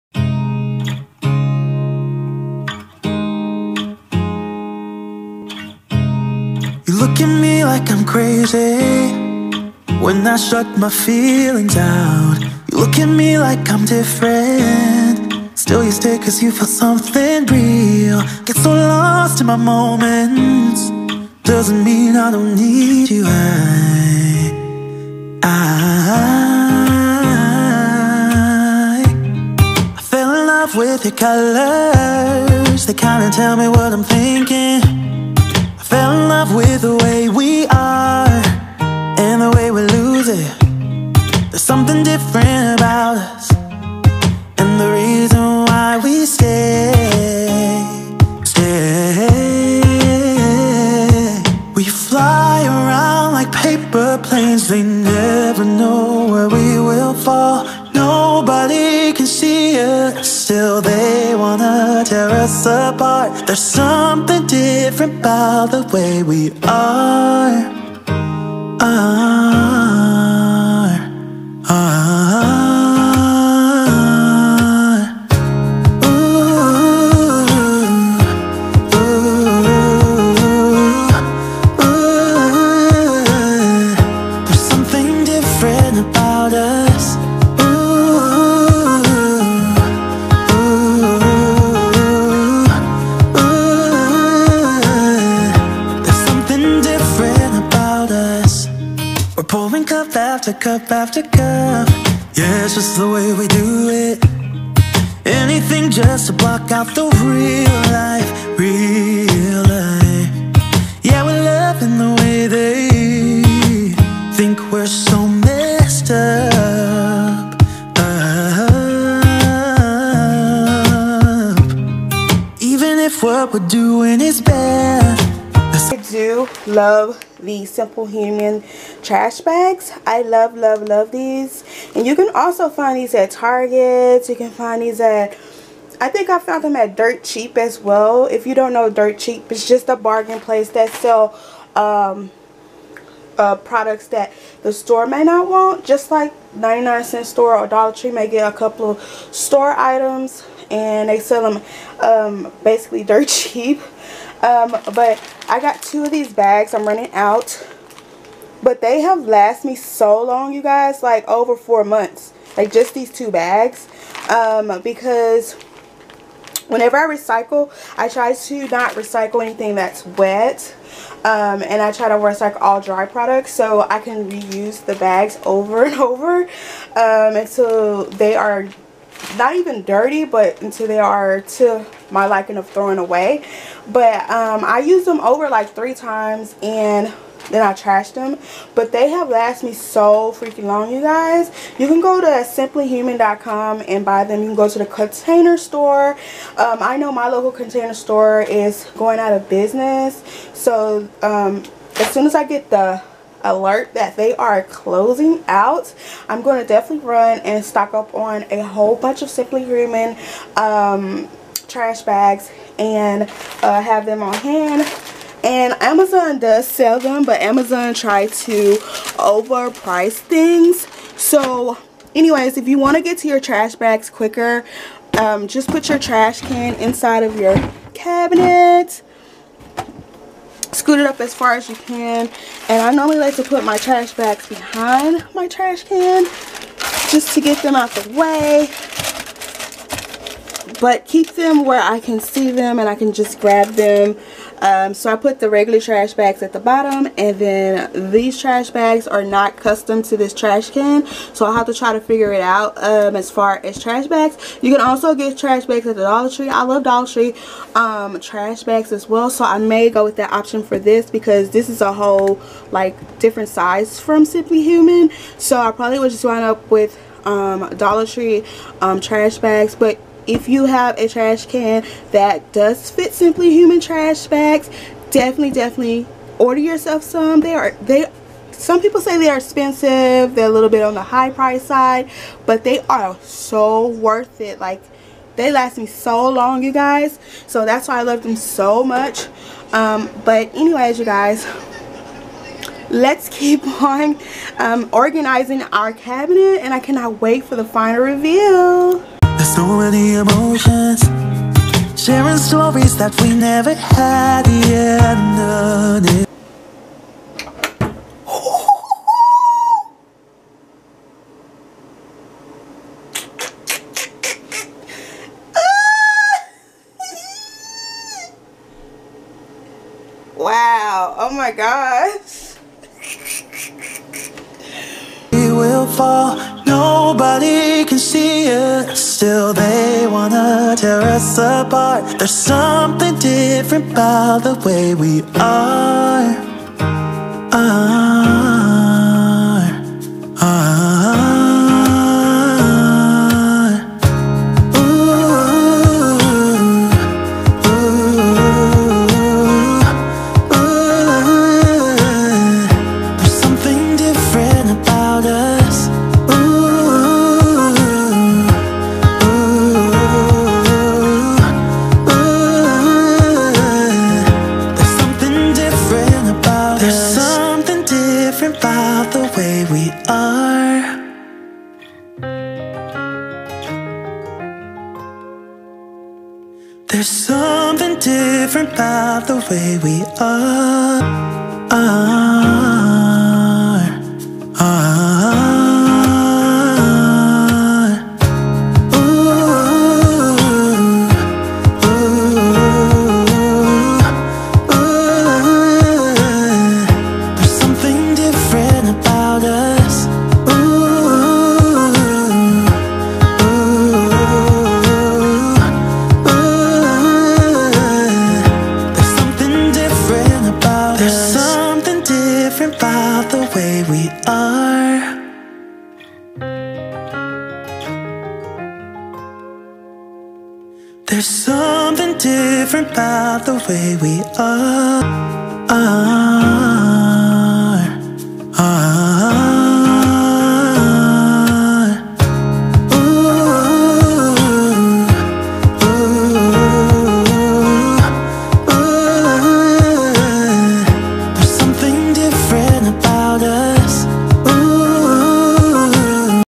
PART2: ORGANIZING & CLEANING UNDER MY KITCHEN SINK CABINET | IT'S ANDREA MICHELL SIS
VLOGMAS 2020 IS CANCELED Y'ALL I WILL SHARE WHY ON A LATER VLOG SIS

FREE PRODUCT REVIEWS ONLY